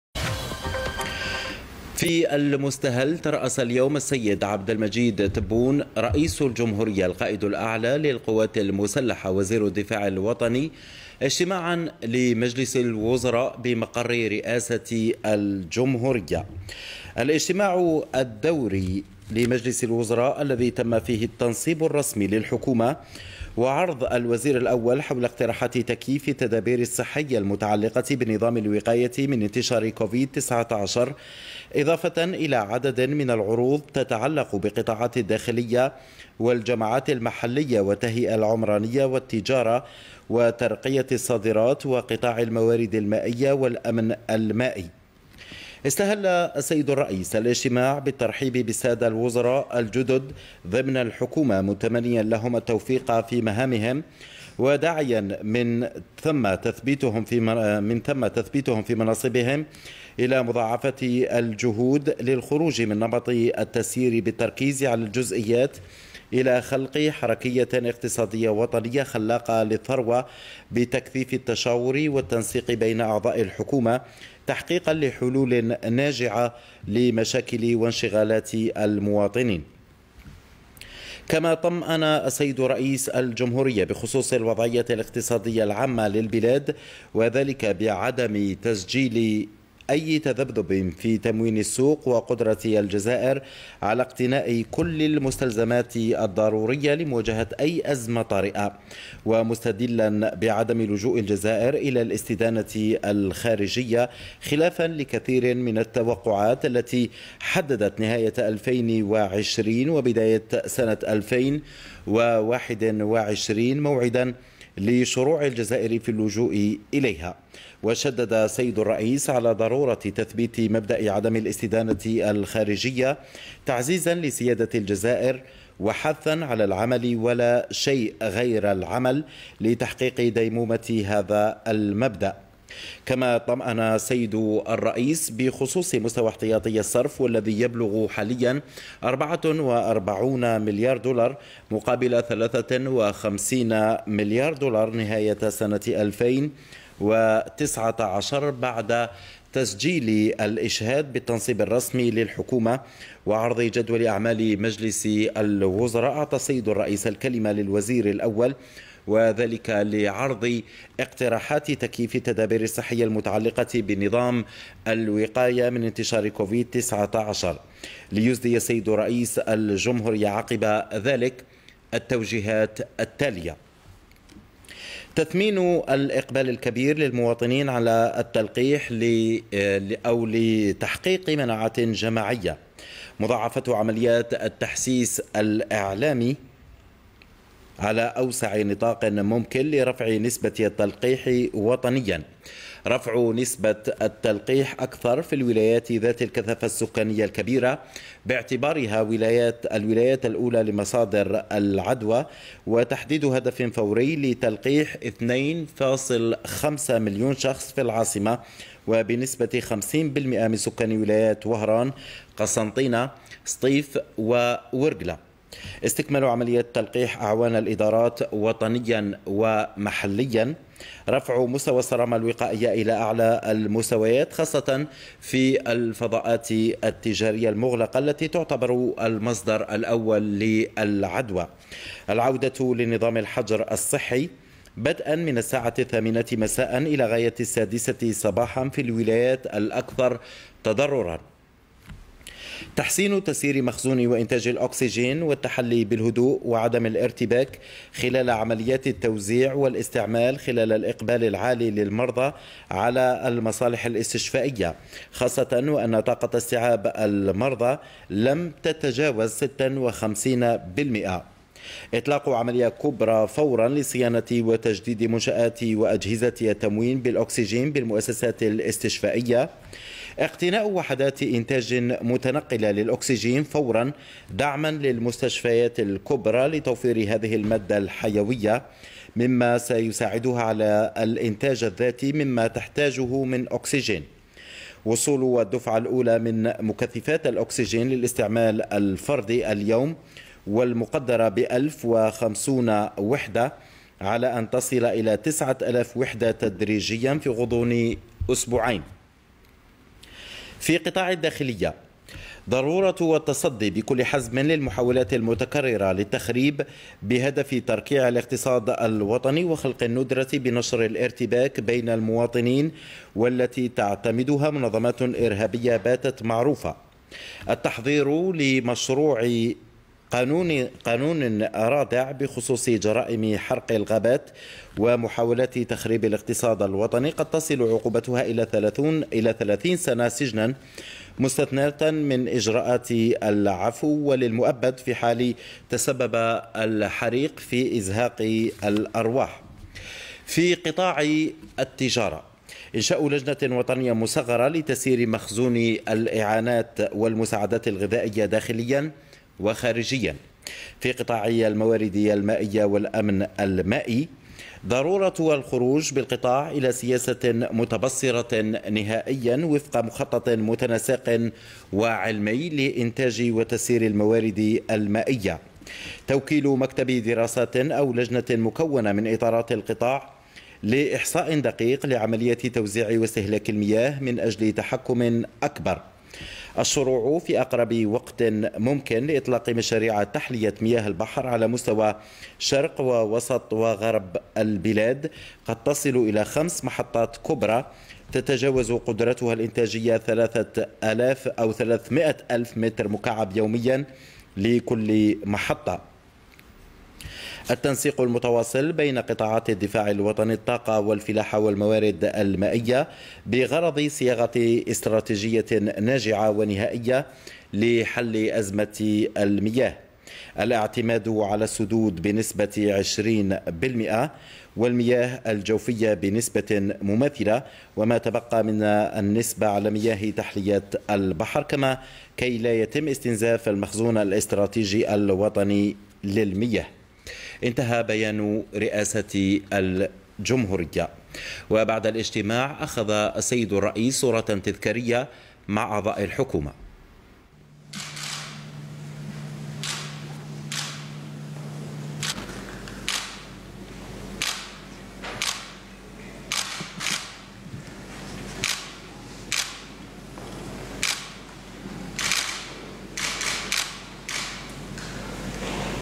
الرئيس تبون يأمر بالعودة لنظام الحجر الصحي من الثامنة مساءً إلى السادسة صباحًا
رئاسة/ السيد عبد المجيد تبون رئيس الجمهورية القائد الأعلى للقوات المسلحة وزير الدفاع الوطني يترأس إجتماعا لمجلس الوزراء
رئيس الجمهورية يأمر بالعودة لنظام الحجر الصحي من الثامنة مساءً إلى السادسة صباحًا في الولايات الأكثر تضررًا

🔴الرئيس تبون يشدد على تثبيت مبدأ عدم الاستدانة الخارجية
🔴الرئيس يطمئن بخصوص مستوى احتياطي الصرف
🔴احتياطي الصرف بلغ  44 مليار دولار